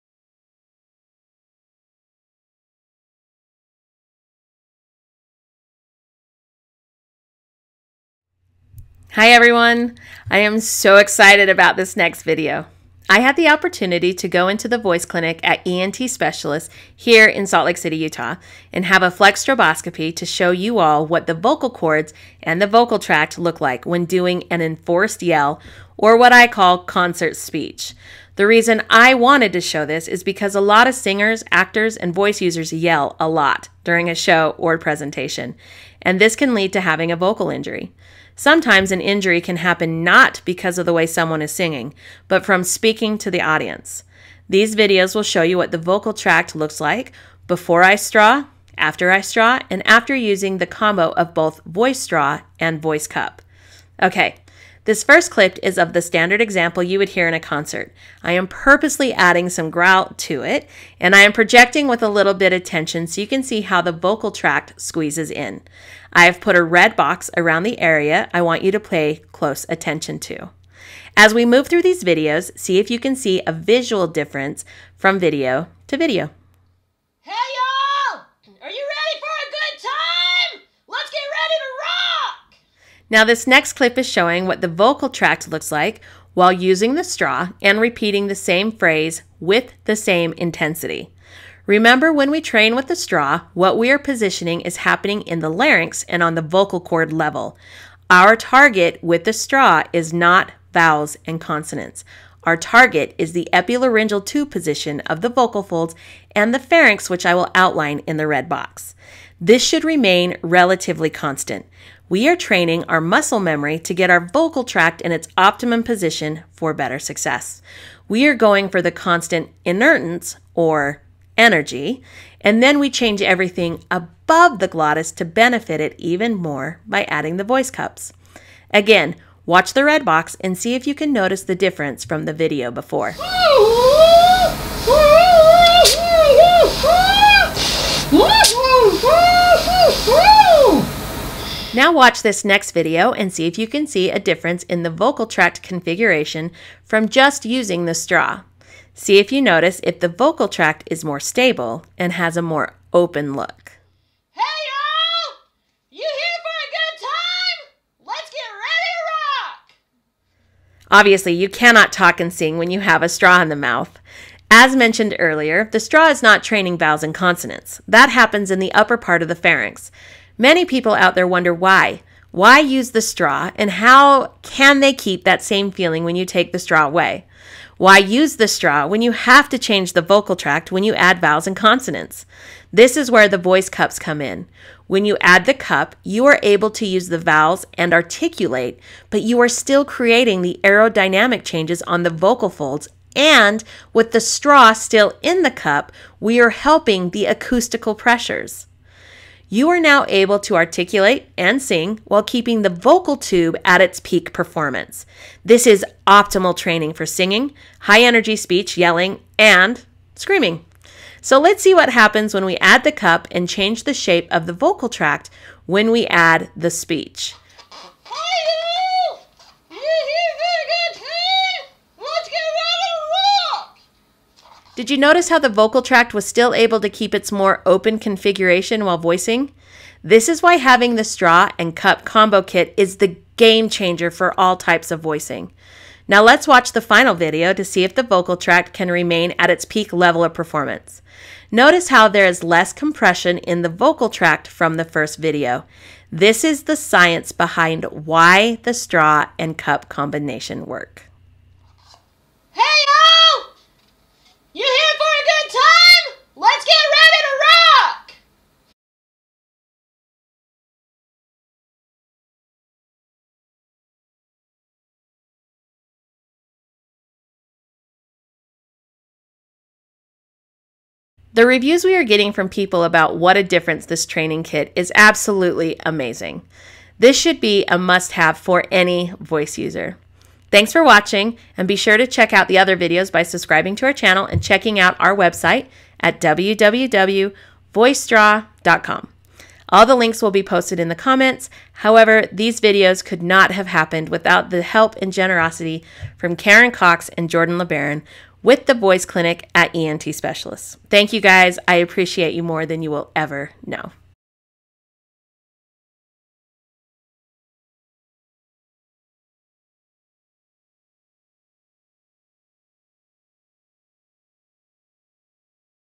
THE VOICE STRAW- Straw phonation showing Enforced Yell
Watch and learn how straw phonations works and why adding the voice cups changes the game! This is an inside look of the vocal tract while doing straw phonation with The Voice Straw and Cups Combo Kit.

This video is specifically shows an enforced yell or "concert speech". See actual before and after videos and still shots.

Special thanks for consultation with the Voice Center of ENT Specialists
and use of the HD flexible distal-tip-chip videostroboscopy equipment
Karin Titze Cox, MA CCC/SLP Vocologist/Clinic Director &
Jordon LaBaron, MM MS CCC/SLP, Senior Vocologist